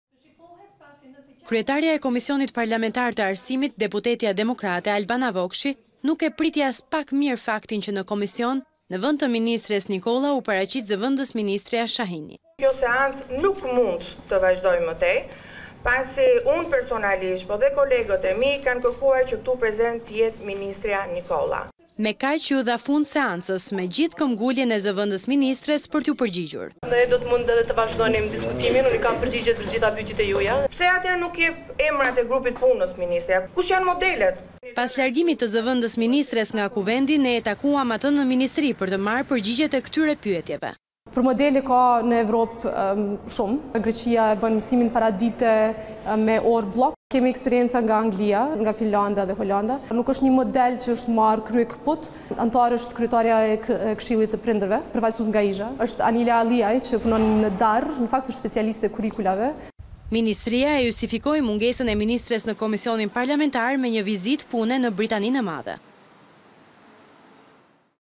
Riorganizmi i lëndëve mesimore - Top Channel Albania - News - Lajme
Kryetarja e Komisionit Parlamentar të Arsimit, deputetja demokrate Albana Vokshi nuk e priti aspak mirë faktin që në Komision në vend të ministres Nikolla, u paraqit zv/ministrja Shahini.